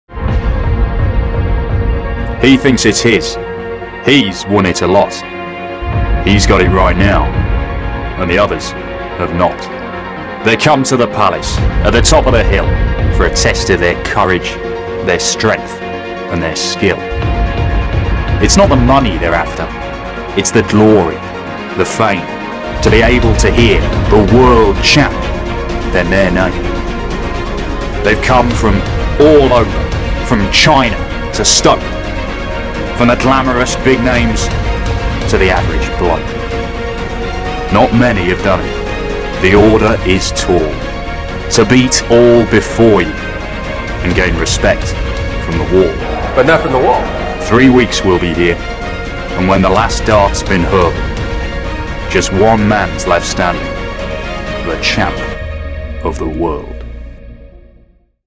15 days of Darting Action... Only one World Champion
If this doesn't get you excited for the William Hill World Darts Championship, I don't know what will!